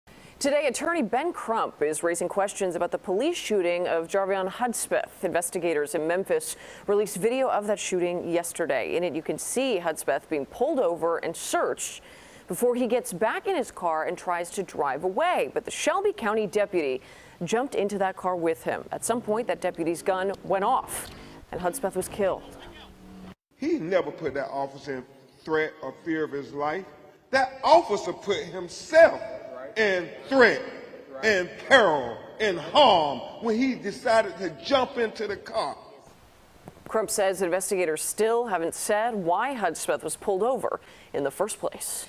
Video released in deputy's fatal shooting of Jarveon Hudspeth
Civil rights attorney Ben Crump is raising questions about the police shooting of Jarveon Hudspeth. Investigators in Memphis have released video of the shooting, which shows Hudspeth being pulled over and searched before he gets back into his car and tries to drive away. A Shelby County deputy jumped into the car with him, and at some point the deputy's gun went off, killing Hudspeth.